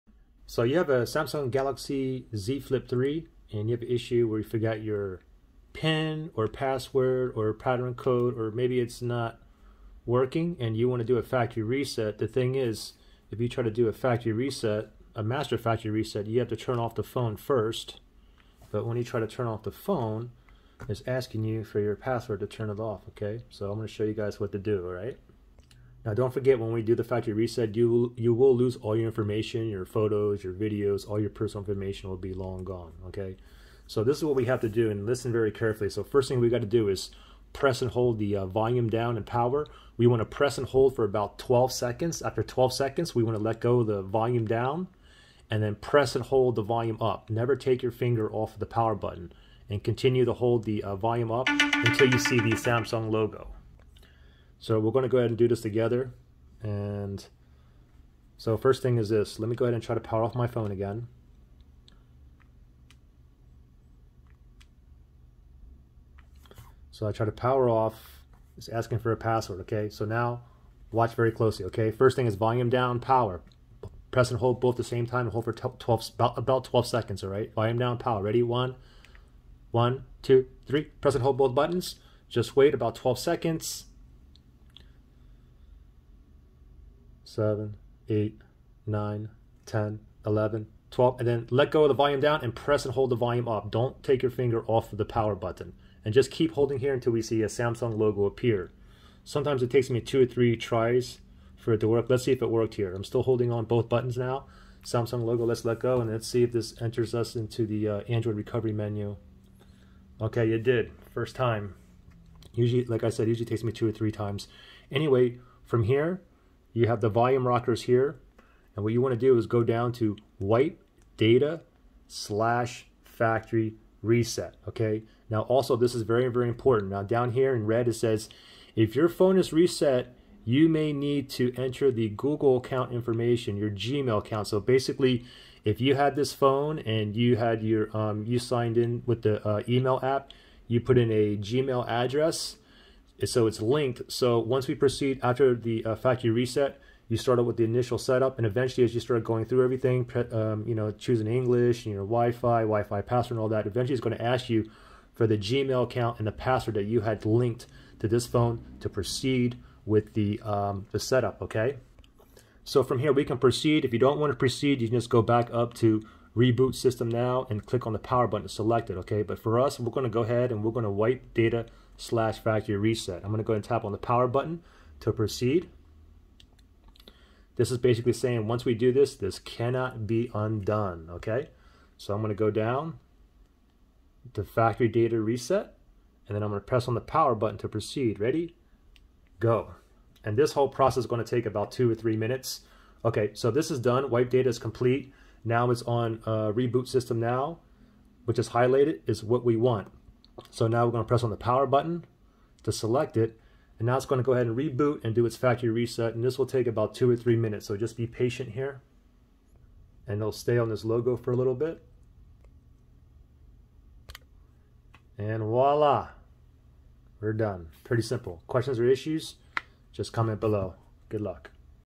Galaxy Z Flip 3 & 4: Forgot Password, PIN, Pattern? Can't Turn OFF to Factory Reset? Watch This!
I show you how to master factory reset a Samsung Galaxy Z Flip 3 smartphone (similar with Galaxy Z Flip 4)where you forgot your password, PIN or pattern or maybe it just doesn't work. Let me know how many times if took you to get into the Android Recovery Options menu. Good Luck!

SAMSUNG Galaxy Z Flip 4